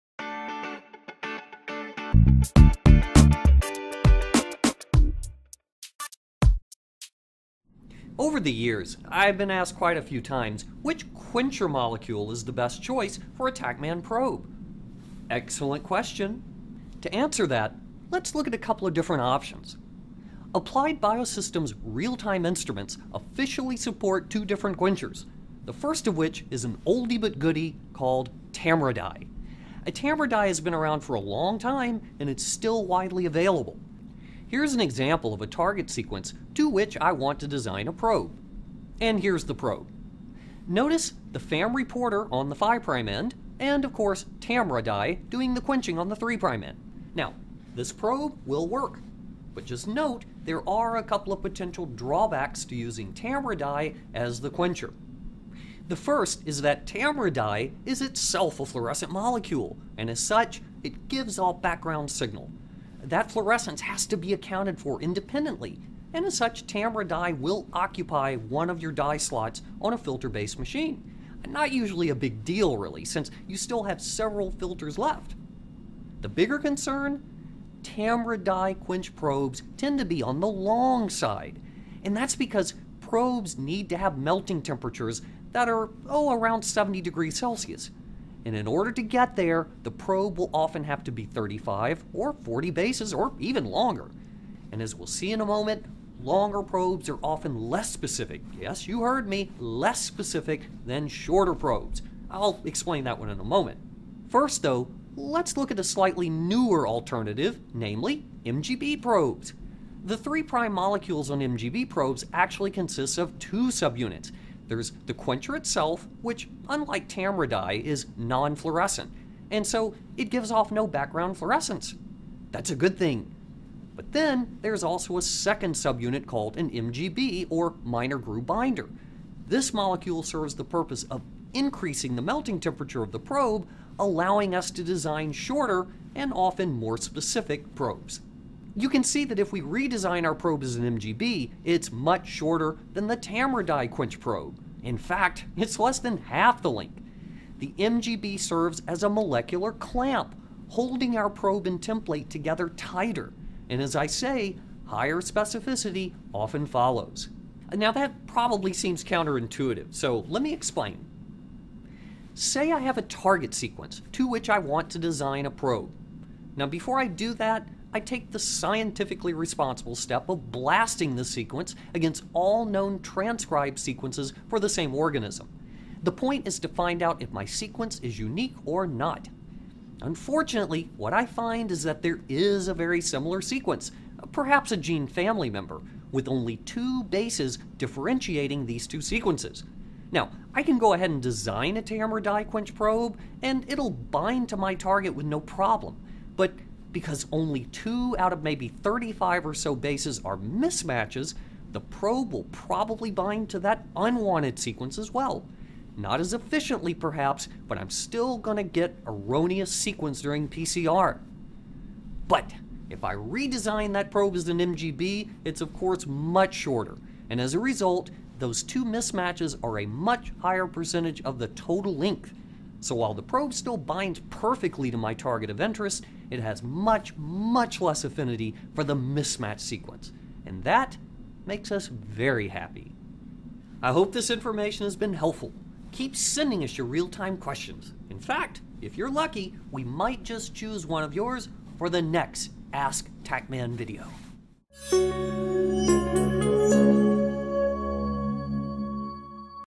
Ask TaqMan ®: Ep. 3 TaqMan® 프로브에는 어떤Quencher분자가 가장 좋을가?
Real-Time PCR에 대한 사용자 여러분의 질문을 등록하시면 라이프 테크놀로지스의Sr. Field Application Specialist인 Doug Rains이 도와드립니다.

[Script]
여러 해 동안 저는 "TaqMan® 프로브에는 어떤 제거물질, 즉 퀜차 분자가 가장 좋은가"라는 질문을 받고 있습니다. 좋은 질문입니다.
우선 어떤 제품을 이용할 수 있는지 살펴보겠습니다. Applied Biosystems® 의 실시간 분석기에는 공식적으로 두 가지 퀜차를 사용할 수 있습니다. 그 중 하나가 오래되었지만 그 품질을 인정받는 TAMRA™ dye입니다. TAMRA™ dye는 오래 동안 사용되어 왔으며 여전히 널리 사용되고 있습니다. 예를 보십시오. 이것은 프로브를 디자인하려하는 표적 시퀀스입니다. 그리고 이것이 프로브입니다.
FAM™ 리포터가 5번 말단에 있지요. TAMRA™ dye가 3번 말단에서 퀜칭을 합니다. 이제 프로브가 기능을 합니다. 그런데 TAMRA™ dye를 퀜차로 사용하는 데는 몇 가지 문제가 있습니다. 첫 번째로 TAMRA™ dye는 그 자체가 형광 분자인데 형광분자는 배경 신호를 발생시킵니다. 이 형광을 따로 측정해야 하기 때문에 필터 기반 기기 염료 슬롯 하나를 TAMRA™ dye에 할당해야 합니다. 하지만 대개 남는 필터가 있기 때문에 별 문제는 아닙니다. 이 보다 큰 문제는 TAMRA™ dye로 퀜칭한 프로브의 길이가 길다는 데 있습니다. 프로브는 섭씨 약 70도 이하의 녹는점을 가져야 하는데 그렇게 하기 위해서는 프로브 길이가 35bp나 40bp 이상이어야 합니다. 잠시 후 보겠지만 프로브 길이가 길면 대개 특이성이 떨어집니다. 네, 짧은 프로브보다 특이성이 낮지요. 이에 대해서는 잠시 후에 설명하겠습니다. 우선 상대적으로 새로운 제품인 MGB 프로브를 살펴봅시다. MGB 프로브의 3번 분자는 실제로 서브유닛 두 개로 이루어집니다. 이게 퀜차지요. TAMRA™ dye와 달리 형광을 띠지 않기 때문에 배경 형광을 발생시키지 않습니다. 좋은 점이죠. 그리고 MGB 즉, minor groove binder 라고 하는 두 번째 서브유닛이 프로브의 녹는점을 높이는 역할을 합니다. 이 때문에 짧으면서 보통 특이성이 더 높은 프로브를 디자인할 수 있습니다. MGB로 좀 전의 프로브를 다시 디자인하면 TAMRA™ dye로 퀜칭한 프로브보다 훨씬 짧아지지요. 절반도 되지 않습니다. MGB는 프로브와 템플릿을 꽉 잡아주는 클램프 역할을 합니다. 그러면 말씀드린 바와 같이 특이성이 높아지게 됩니다. 이 말은 확 와닿지 않을 수 있습니다. 설명하자면 프로브를 디자인하고자 하는 표적 시퀀스가 있다고 합시다. 우선 이 시퀀스를 이와 동일한 유기체에 알고 있는 모든 전사 시퀀스에 대해 과학적으로 BLASTing합니다. 이 시퀀스가 고유한지 알아보는 것이지요. 음, 여기에는 매우 유사한 시퀀스가 하나 있군요. 같은 유전자 계열로 보이는데 염기 2개만 다릅니다.
이제, TAMRA™ dye 퀜치 프로브를 디자인해 봅시다. 문제없이 표적에 결합할 것입니다. 그런데 35개 가량의 염기 중 2개만 다르기 때문에 이 프로브는 표적이 아닌 시퀀스에 결합할 수 있습니다. 그래서 효율성이 떨어지며 PCR 중에 잘못된 시퀀스를 갖게 될 것입니다. MGB로 프로브를 다시 디자인해 봅시다. 물론 길이가 훨씬 짧아지죠. 그렇기 때문에 미스매치 2개는 전체 길이로 봤을 때 그 비율이 커집니다. 그래서 프로브가 표적에는 확실히 결합하며 미스매치 시퀀스에는 친화성이 훨씬 떨어집니다. 좋지요.
이 정보가 도움이 되었길 바랍니다. 저희에게 언제든 실시간 PCR 질문을 보내주세요. 운이 좋다면 다음 시간에 여러분의 질문이 선택될 수도 있습니다.